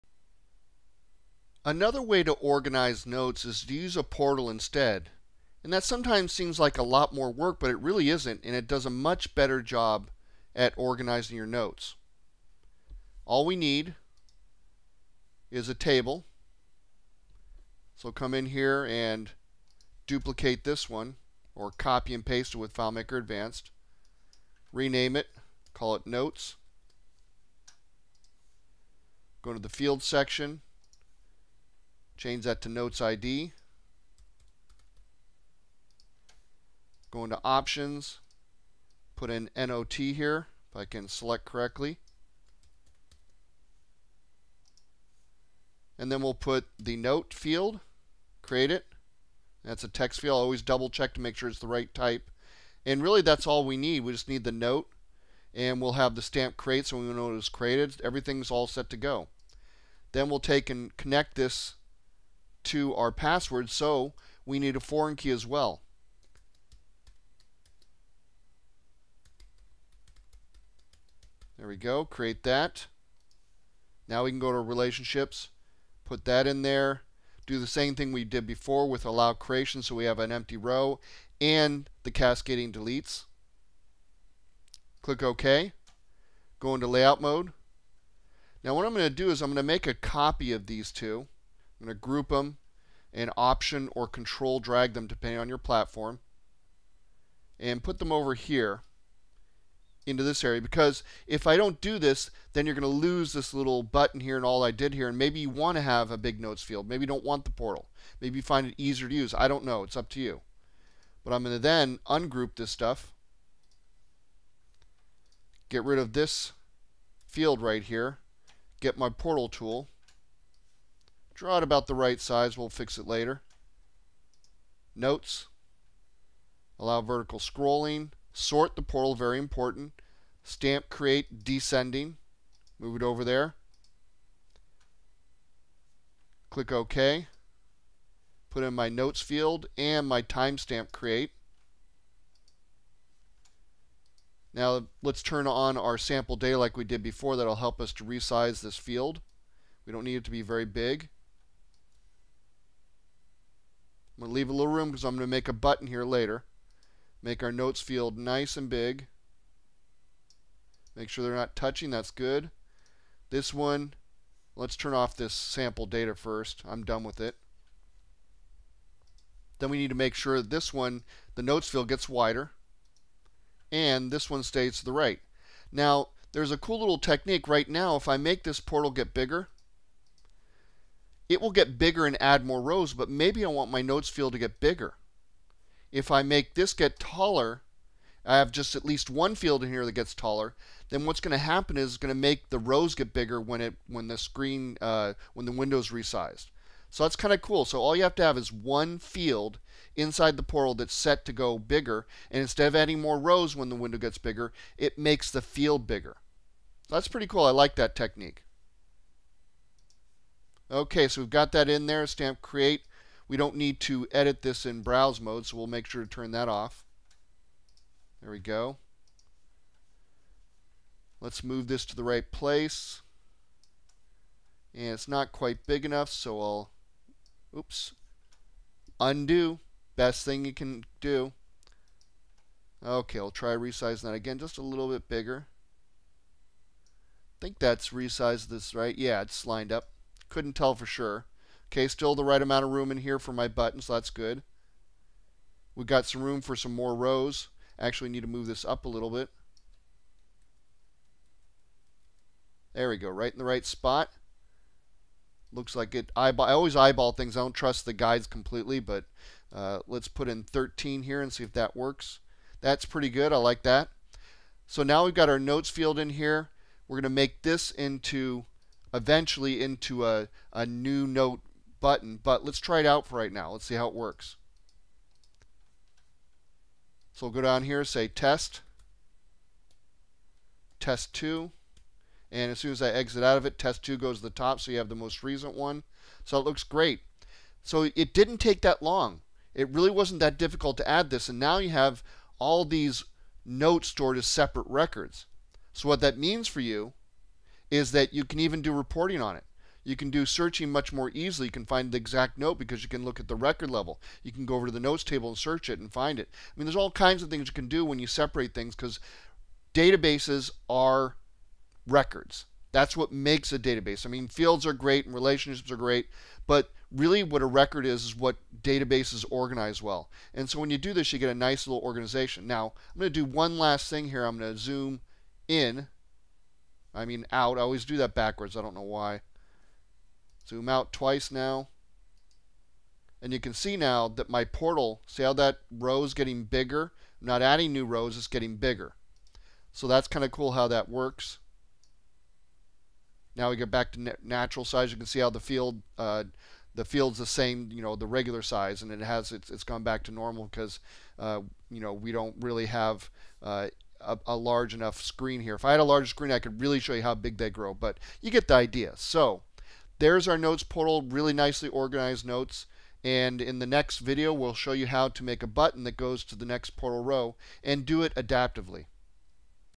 New Portal Row
Dynamically add a new portal row to any FileMaker portal using script parameters.